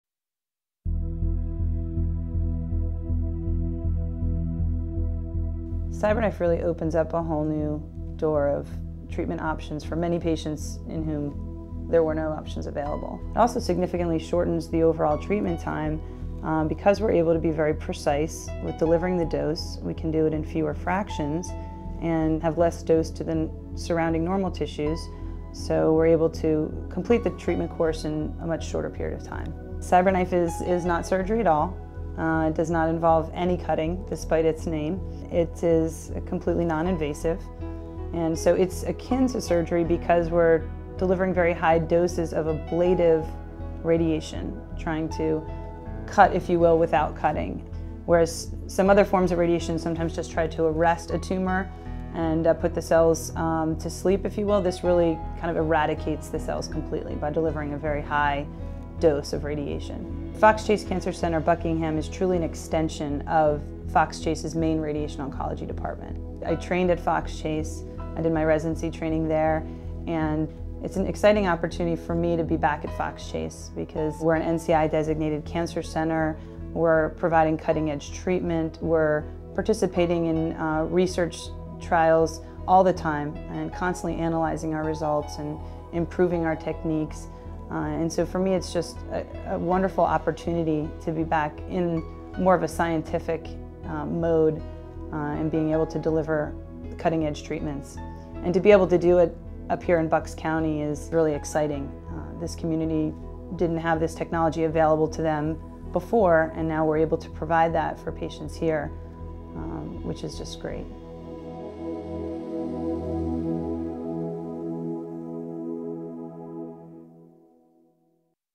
Cyberknife cancer treatment at Fox Chase Buckingham
Dr. Shelly Hayes, radiation oncologist at Fox Chase Cancer Center - Buckingham, explains how Cyberknife technology is used to eradicate cancer cells, while sparing healthy, nearby tissue. This non-invasive treatment offers high dose treatment in a shorter amount of time. Cyberknife is currently available for Fox Chase patients in the Buckingham, Pennsylvania office.